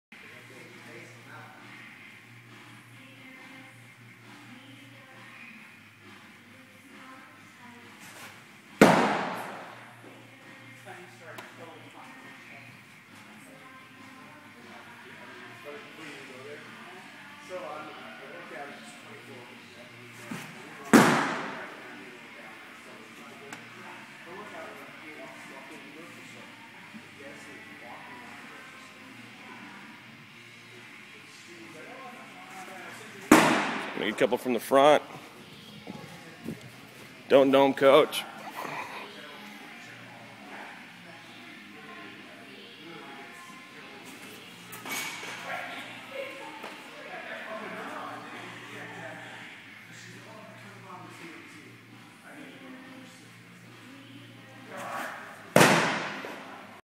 Max Gross Pitching 9/28/11
Untouched mechanics at the start of lesson 1.